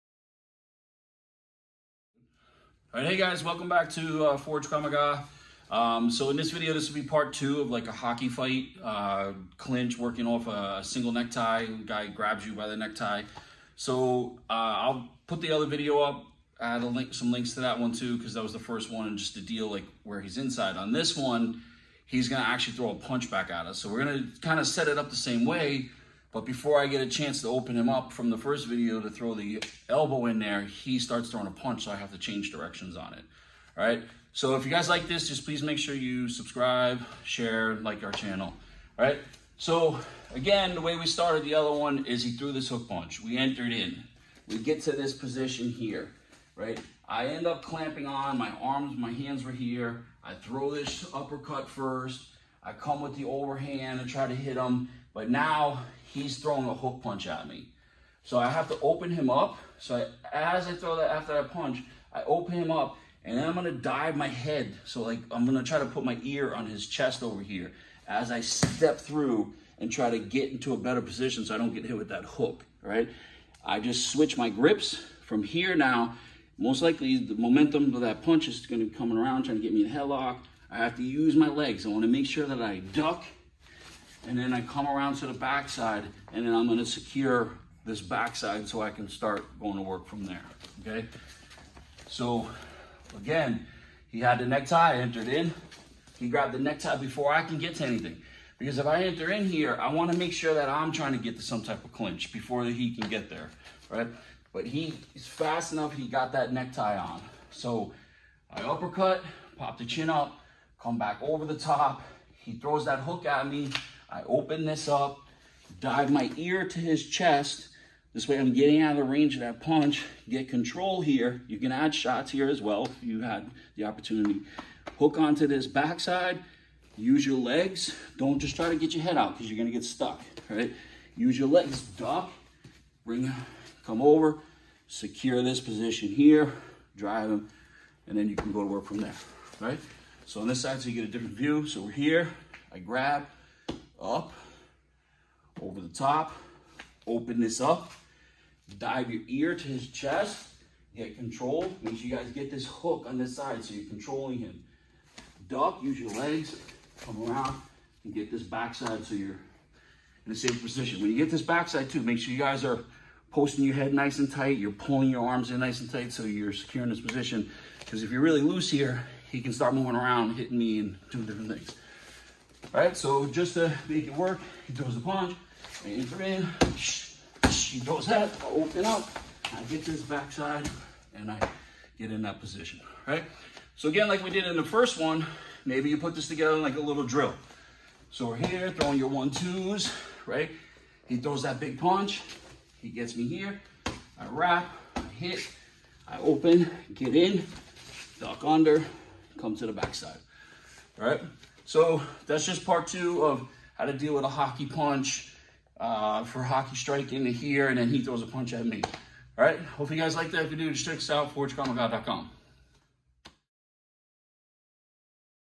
Hockey Clinch Dirty Boxing Part 2 - Forged Krav Maga Self Defense
Hey guys, in this video I’m going over how to deal with a single neck tie. Hockey fighting for Clinch Dirty Boxing. This is Part 2 where he grabbed your neck with a single neck tie and tries to hit you back. This is a great drill to mix in with live boxing.

Forged Strong. Fight Ready.